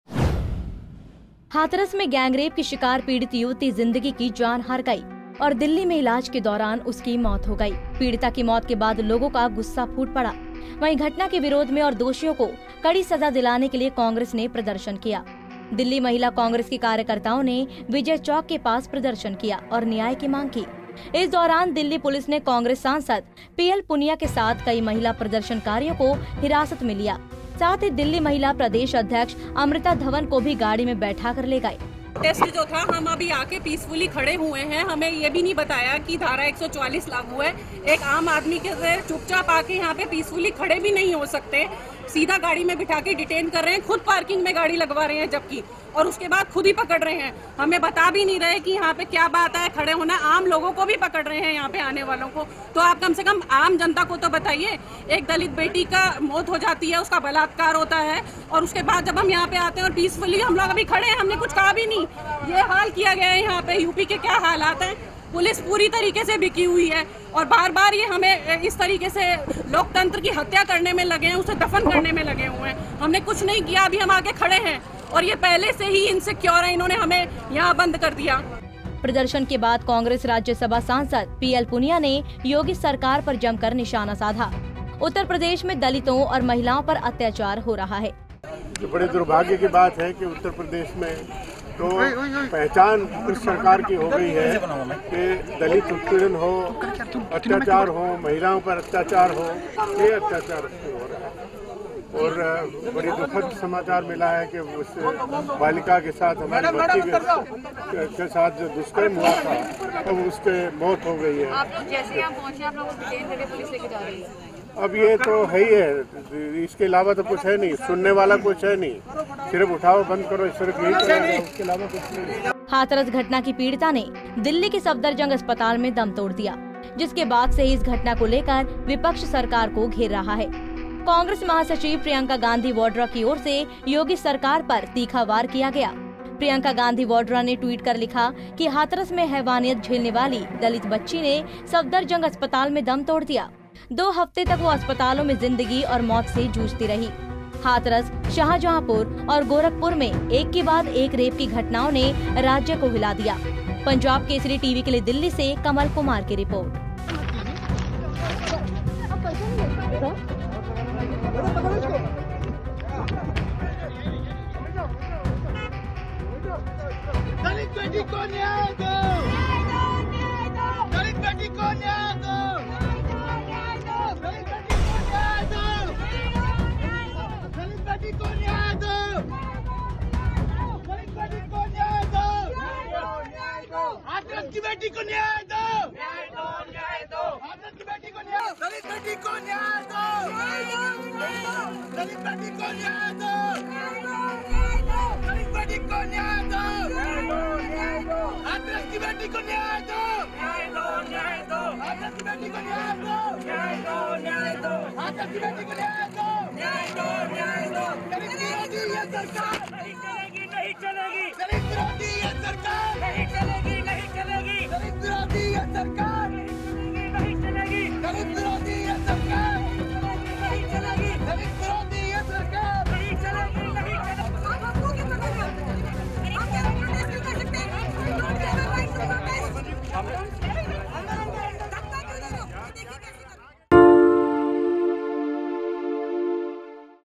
हाथरस गैंगरेप: विजय चौक पर महिला कांग्रेस का हल्ला बोल, कई नेताओं को हिरासत में लिया गया
हाथरस में गैंगरेप की शिकार पीड़ित युवती जिंदगी की जान हार गई और दिल्ली में इलाज के दौरान उसकी मौत हो गई. पीड़िता की मौत के बाद लोगों को गुस्सा फूटा पड़ा...वहीं घटना के विरोध में और दोषियों को कड़ी सजा दिलाने के लिए कांग्रेस ने प्रदर्शन किया...दिल्ली महिला कांग्रेस की कार्यकर्ताओं ने विजय चौक के पास प्रदर्शन किया और न्याय की मांग की...इस दौरान दिल्ली पुलिस ने कांग्रेस सांसद पीएल पूनिया के साथ कई महिला प्रदर्शनकारियों को हिरासत में लिया...साथ ही दिल्ली महिला प्रदेश अध्यक्ष की अध्यक्ष अमृता धवन को भी गाड़ी में बैठाकर ले गए.